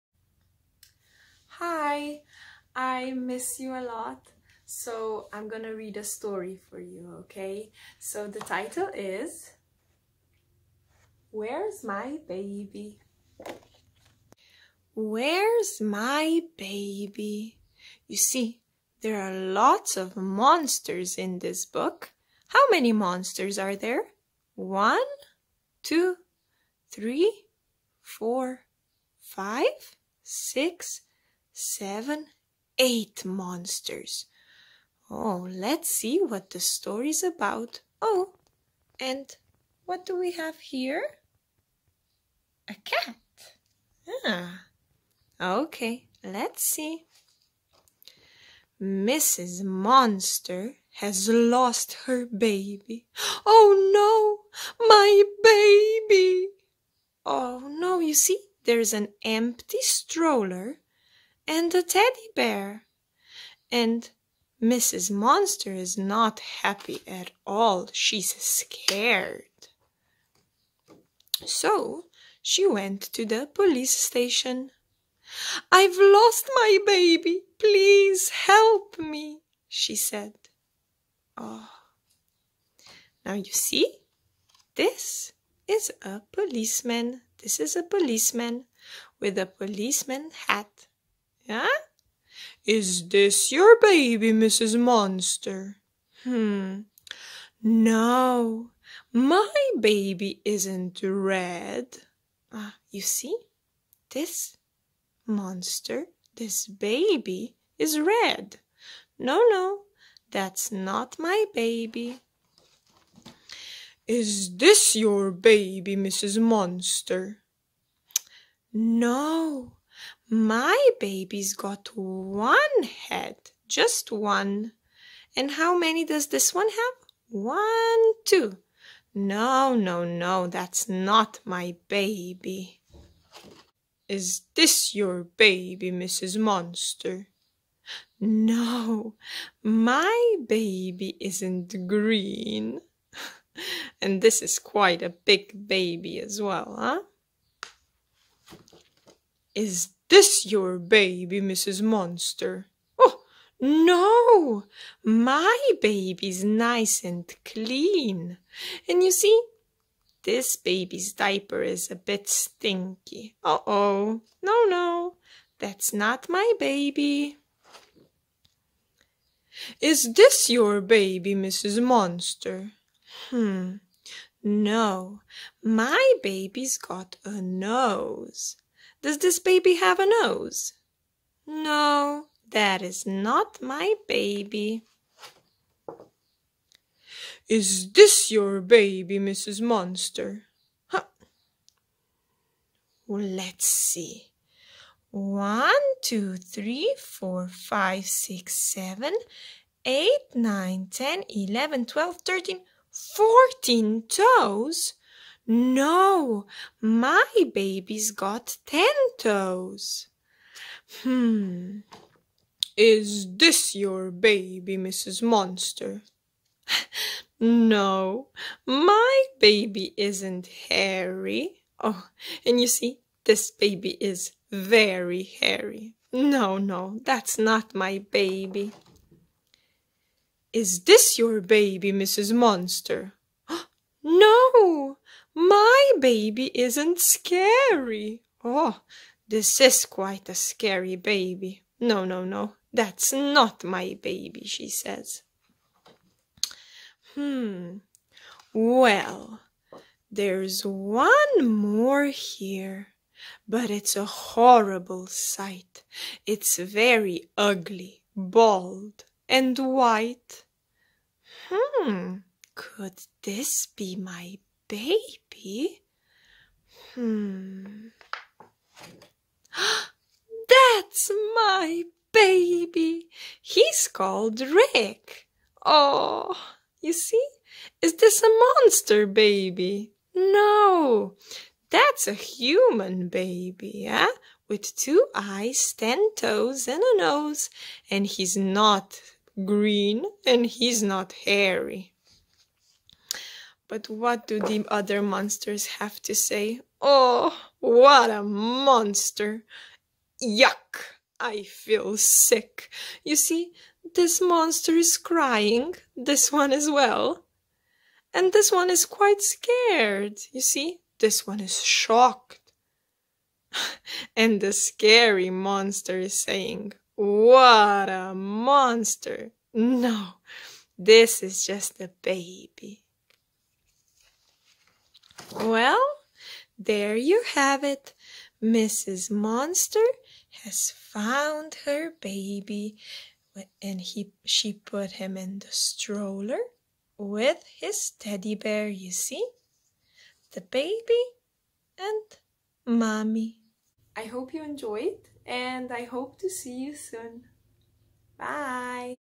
Ana's Storytelling: Where's My Baby
Teacher Ana reads a story to revise colours, numbers and body parts.